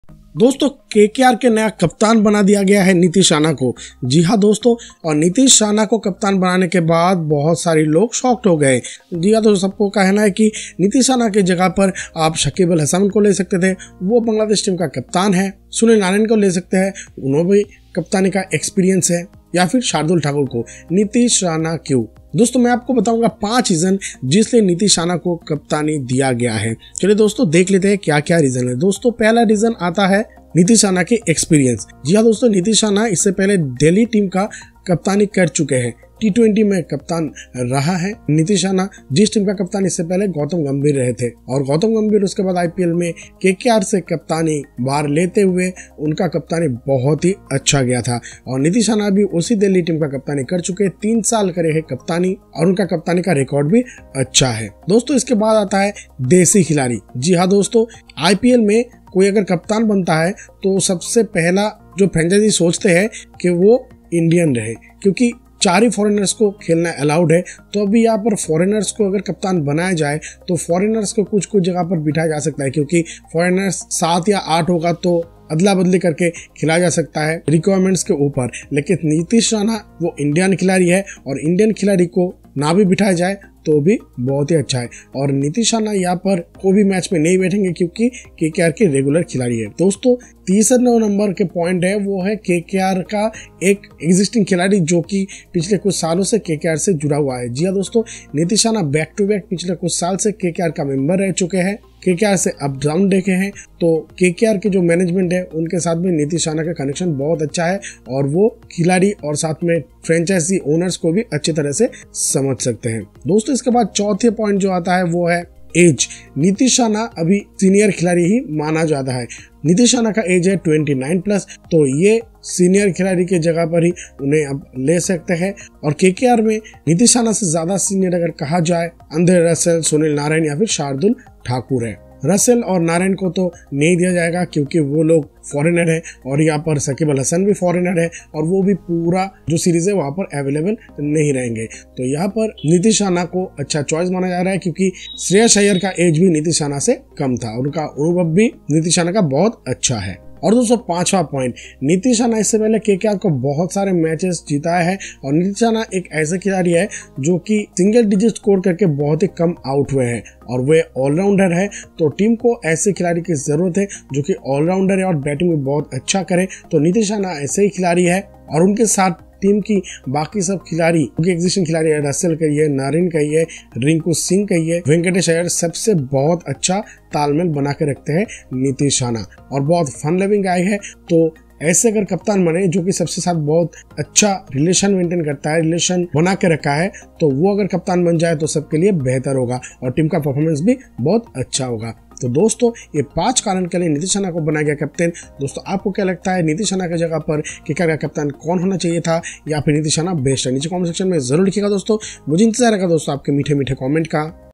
5 Reasons why Nitish Rana became captain of KKR for IPL 2023
5 Reasons why Nitish Rana became captain of KKR for IPL 2023